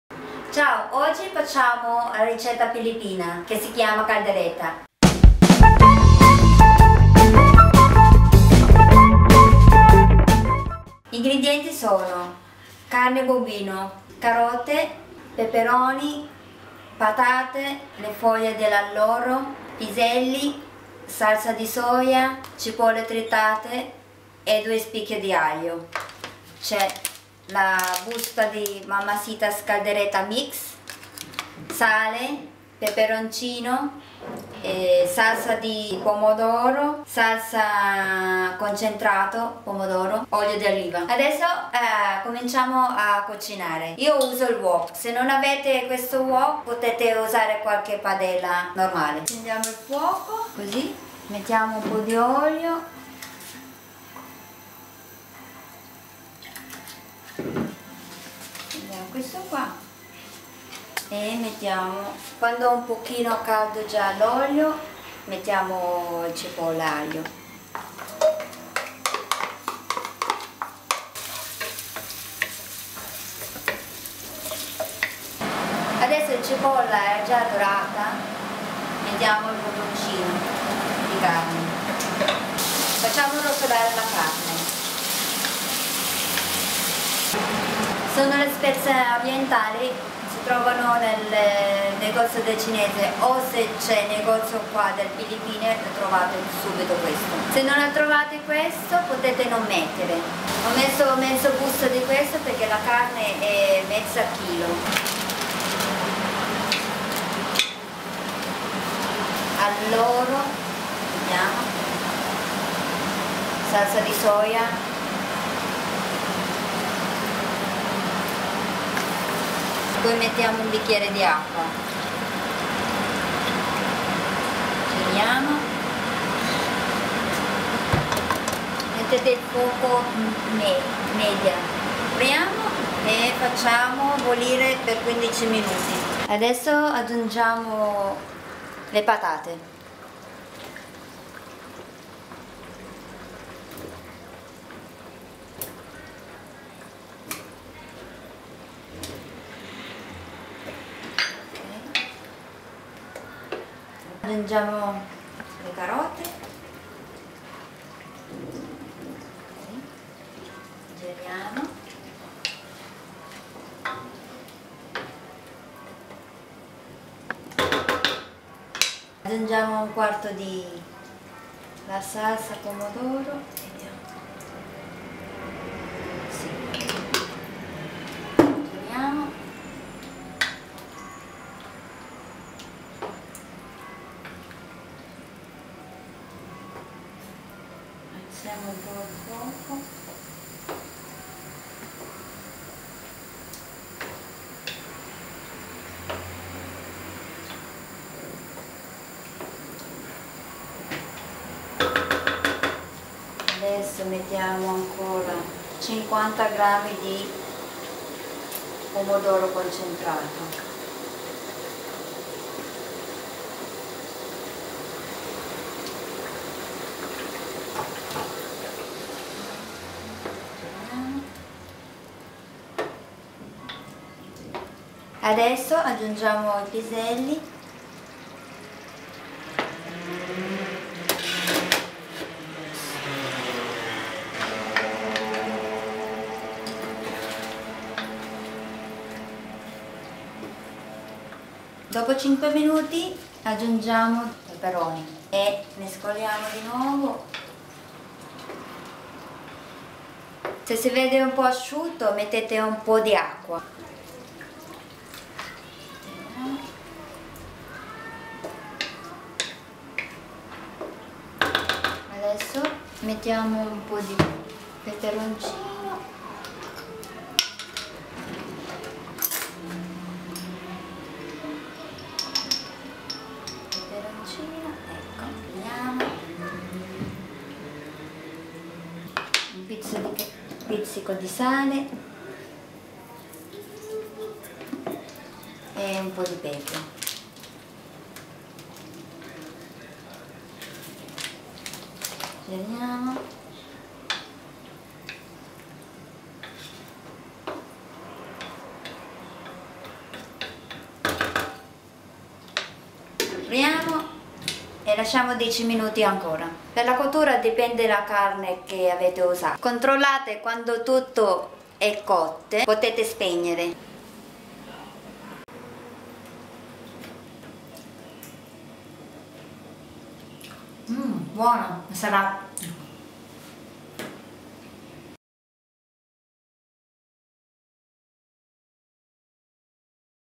Kaldereta (ricetta Filippina)
Ricetta Filippina, si può fare con carne di bovino, vitello o maiale.